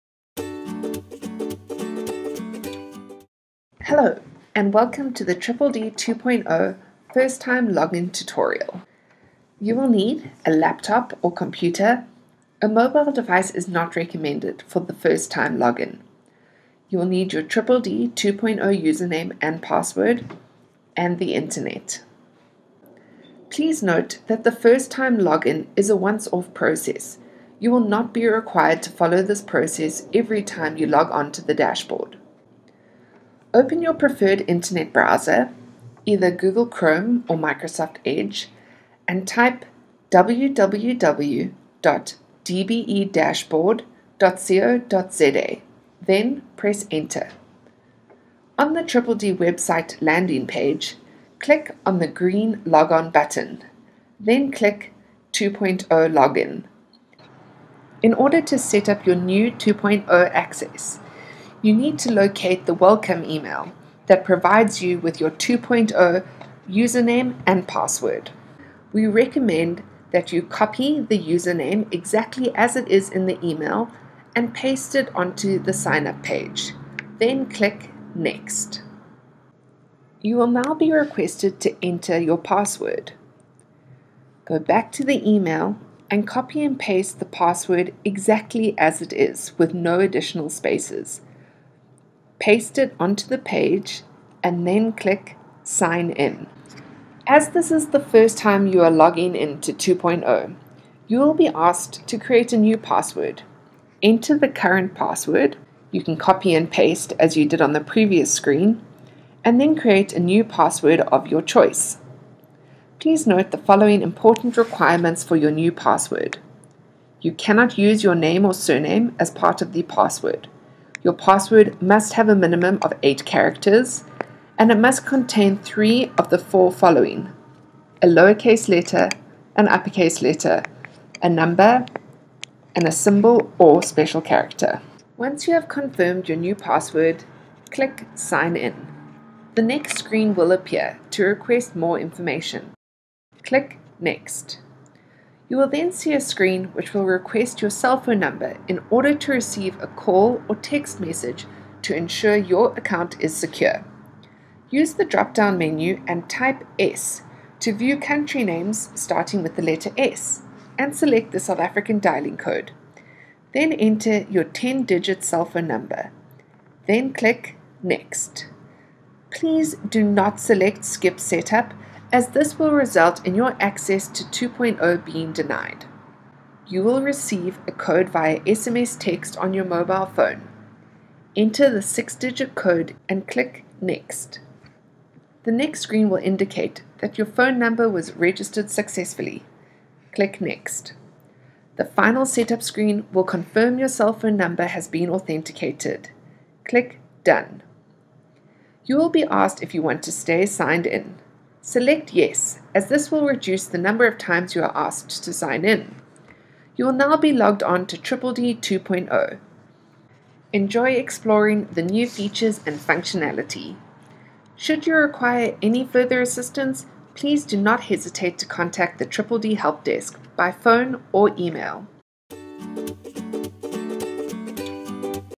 DDD 2.0 first time login tutorial
This video tutorial demonstrates how to log on to DDD 2.0 for the first time.